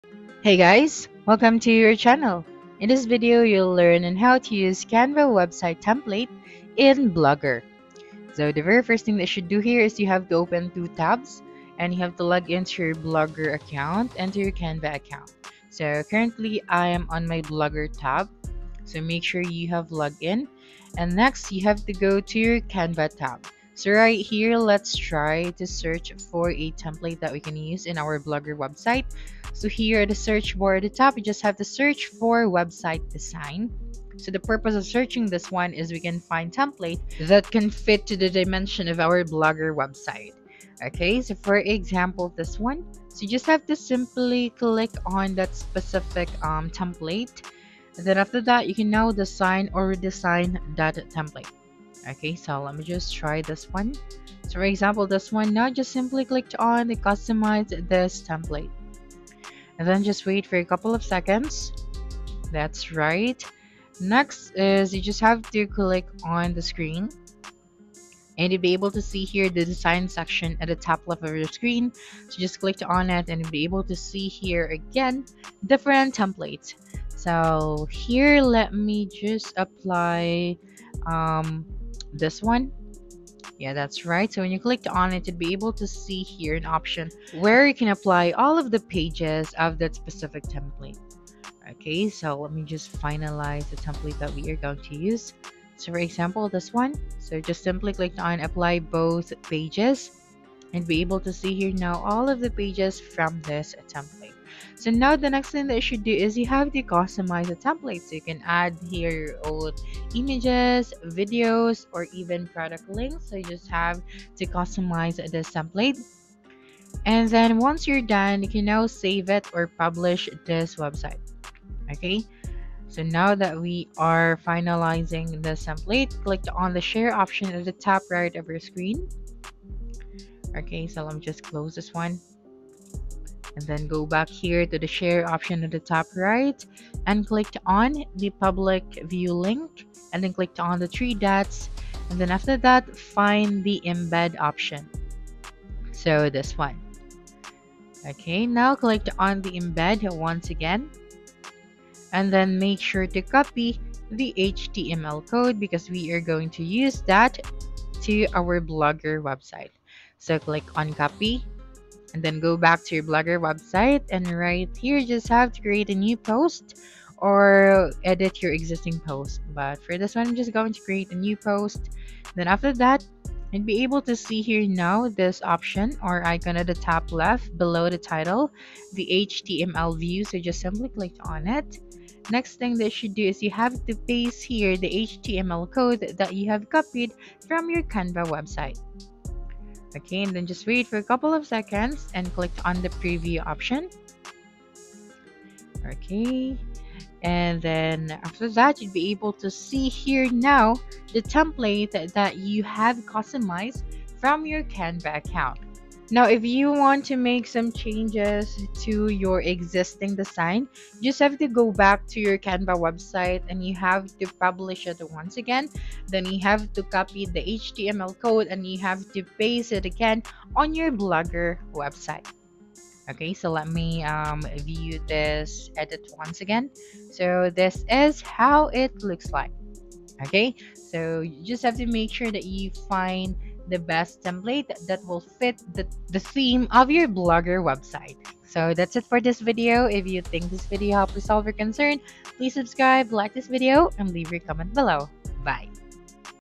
How To Use Canva Website Template In Blogger (Best Method)
Learn the best method to use Canva website templates in Blogger in this easy-to-follow tutorial. Boost your blog design with these simple steps!